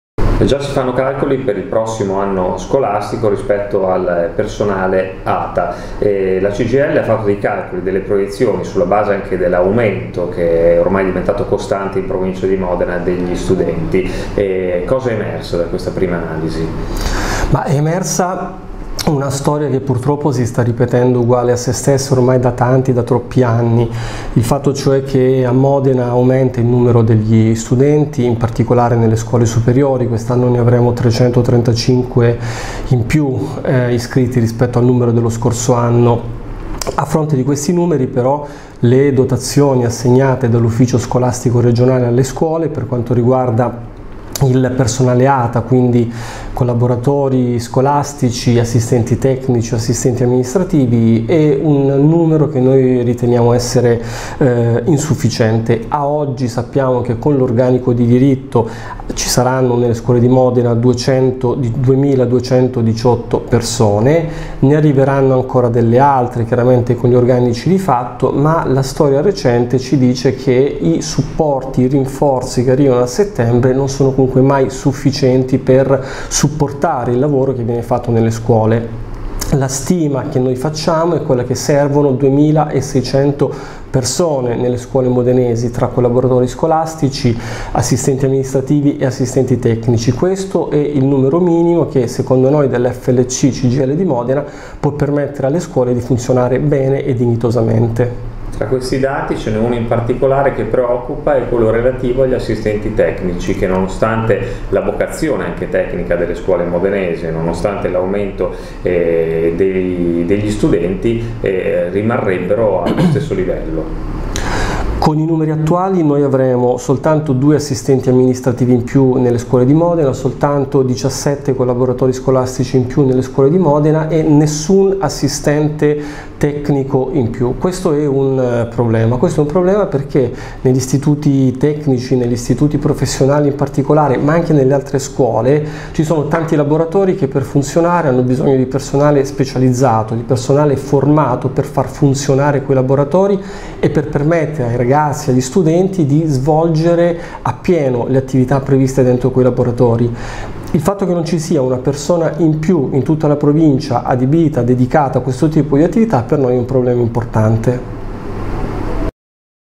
335 studenti in più, il personale ATA non sarà sufficiente
Gli studenti iscritti agli istituti scolastici di ogni ordine e grado aumenteranno ancora di 335 unità. In base alle previsioni della CGIL  scuola di Modena il personale ATA attualmente previsto non consentirebbe di coprire il fabbisogno